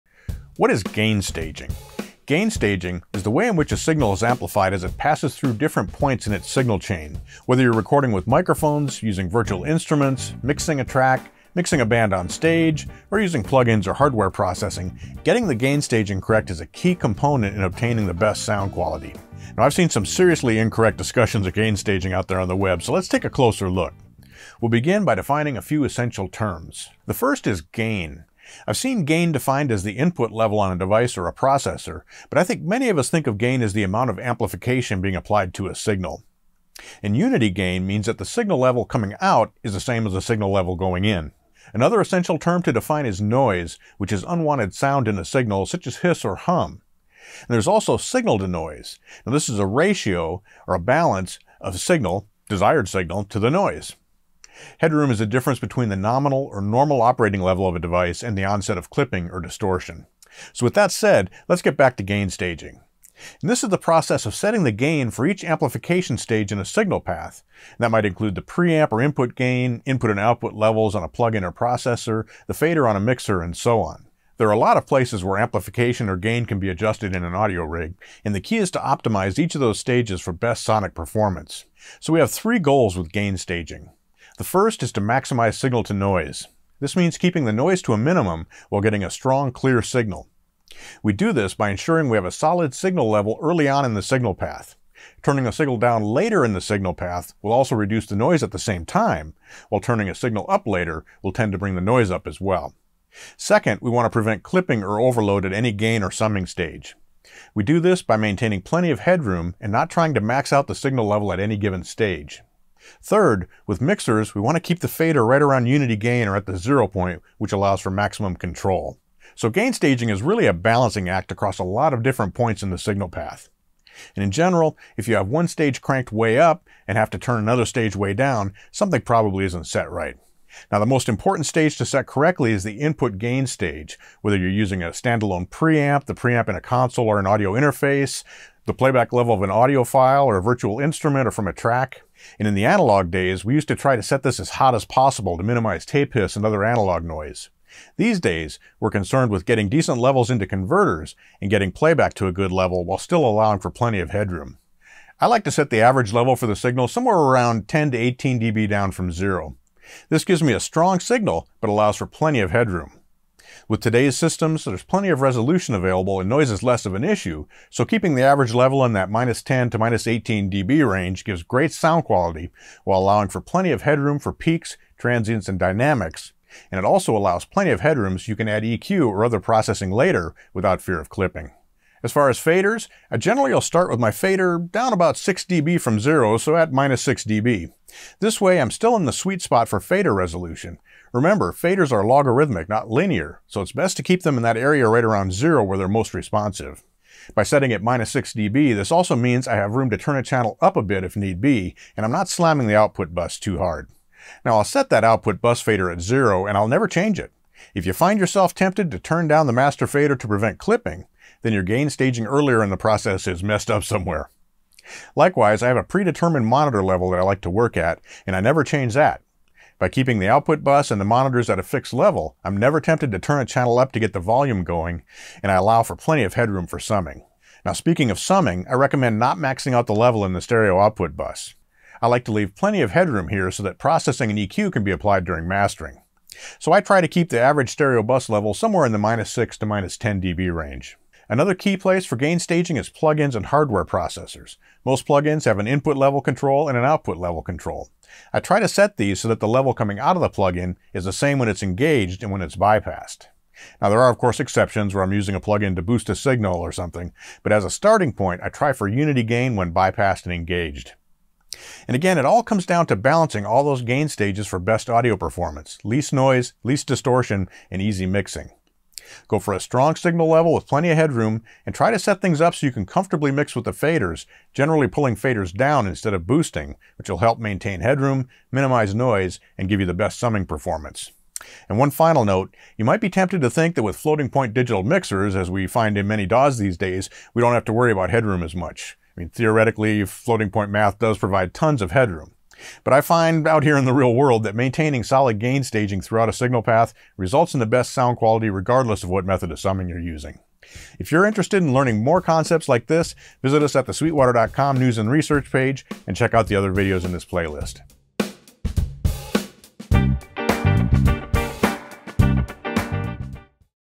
What Is Gain Staging?
If you‘re wondering what video gear we use, check it out

Proper gain staging is critical to achieving the best sound quality possible. That said, many forums and message boards leave much to be desired when trying to fully understand what gain staging is and how to dial in proper settings on your gear. In this video, Sweetwater‘s Mitch Gallagher talks through gain staging and the different ways it presents itself in production and live sound. Check it out!

After you watch, check out Sweetwater.com today for all your music instrument and pro audio needs!